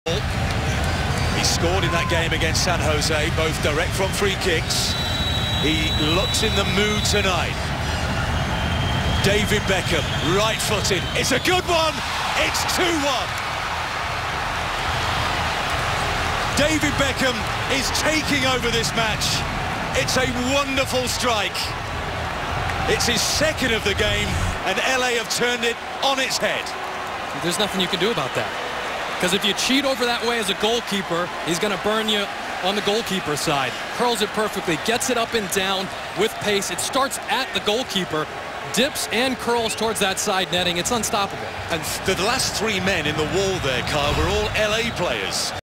This is David Beckham's signature curved free kick
David Beckham sends one into the back of the net, curling it up and over the wall to give the LA Galaxy a 2-1 lead over the Portland Timbers in the 24th minute of their match on July 14, 2012.